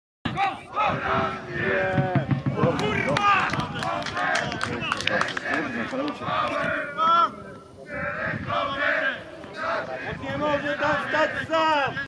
Sęp - Wektra Zbuczyn: okazja bramkowa Przemysława Jasińskiego
Około 25. minuty spotkania goście mieli doskonałą okazję do zdobycia gola. Przed szansą trafienia stanął Przemysław Jasiński jednak posłał piłkę ponad bramką.